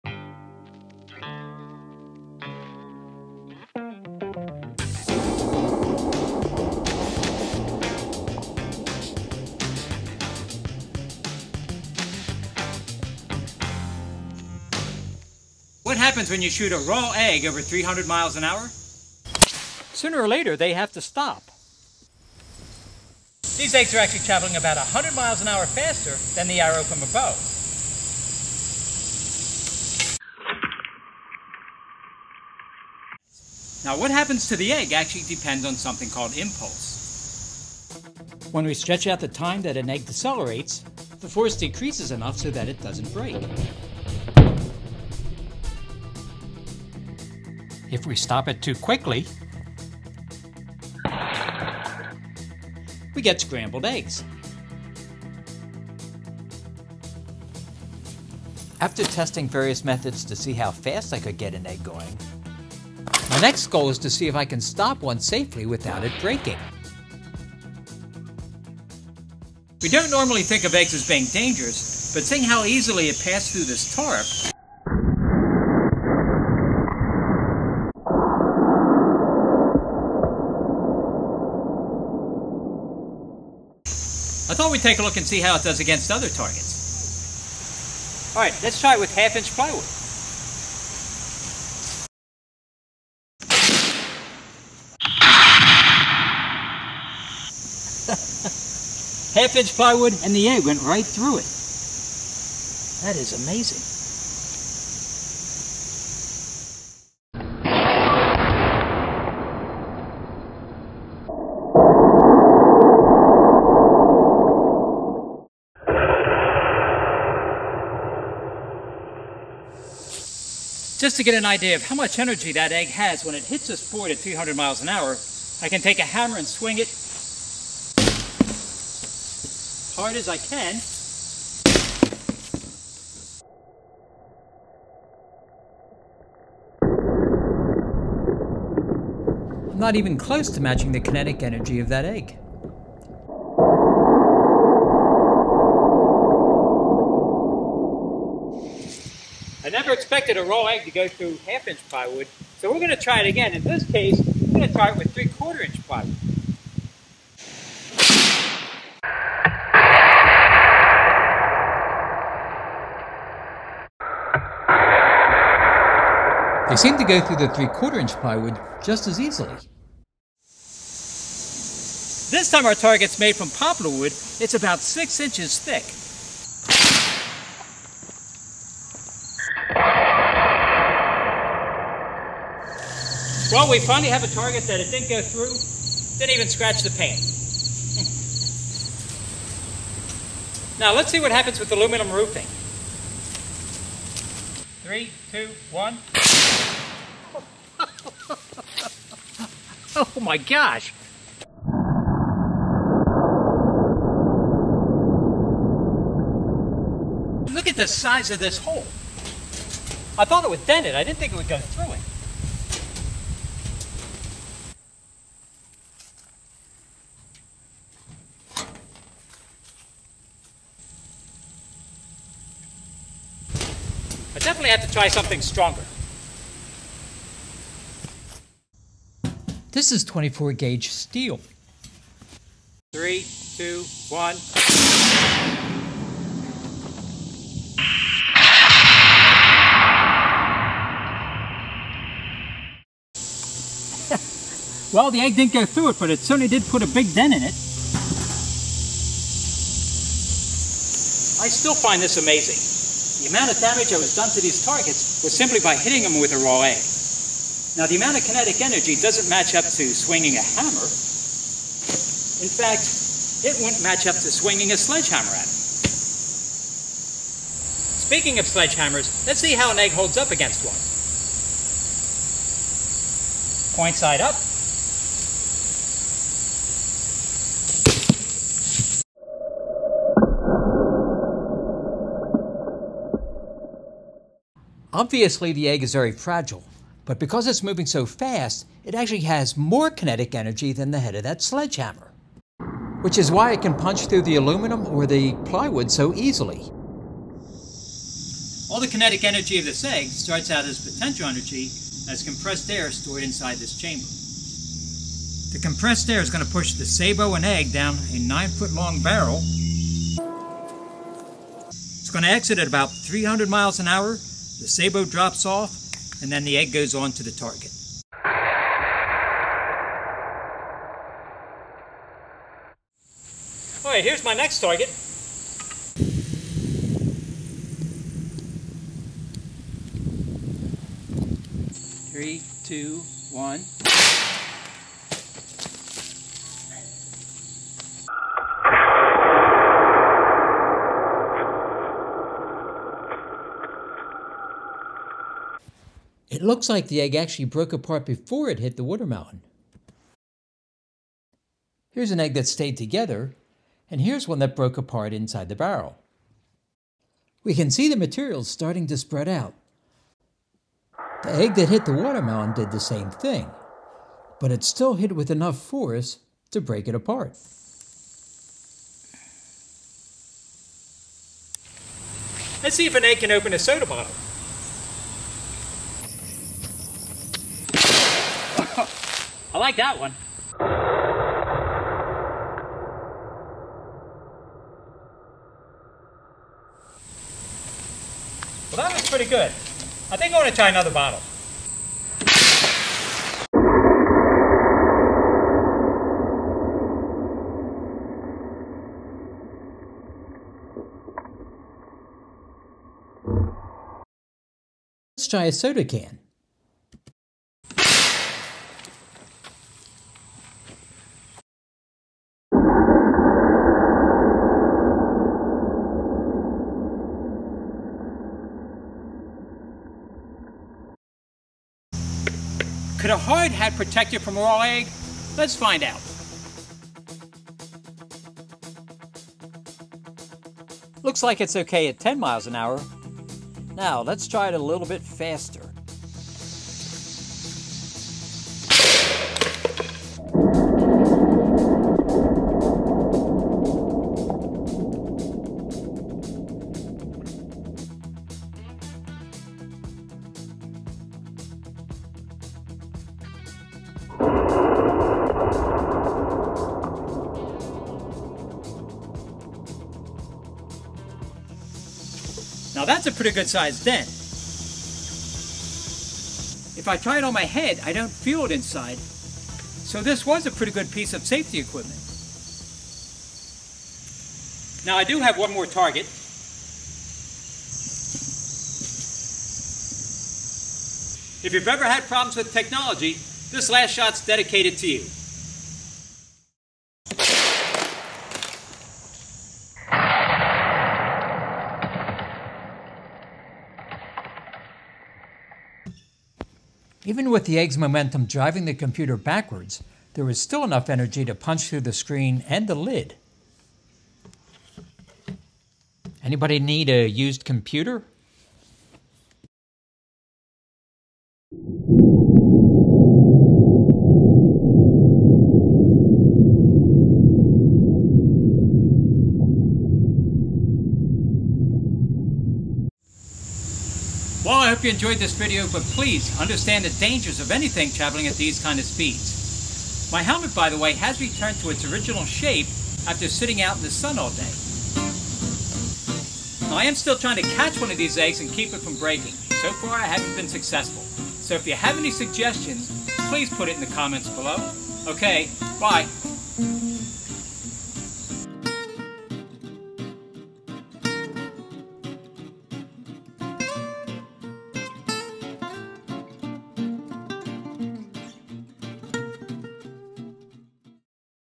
Egg Target Practice at 300 mph (483 km/h)
After figuring out how to launch raw eggs to over 300 mph( 483km/h),   I thought I'd have a little fun  with the cannon and  see what can withstand getting hit by a flying egg. It's surprising just how much energy they have at high speeds and the amount of damage they can do. At 300+ MPH even something as fragile as an egg could be dangerous and requires some respect for the amount of energy it has.  Video shows good example of impulse

PVC AIr cannon  dangers future episode:  I am looking for information on  safety problems that anyone has had with PVC cannons.

this video is a follow up to  Launching Raw eggs

A future video will feature attempts to try and stop a 300 mph  egg safely without it breaking

For anyone wondering about wasting food, the eggs were supplied  and were  past their sellable expiration date

.Okay so I've already received offers for it,  selling  the damaged computer listing was supposed to be a joke,  not only is the screen destroyed but it also has raw egg and water inside the body.  I don't think any amount of repairs will bring it back to life

.One last note: the high pitched sound that is heard throughout the video is due to seasonal male cicadas making a loud buzzing noise to attract a mate. A problem when trying to shoot video outdoors during the summer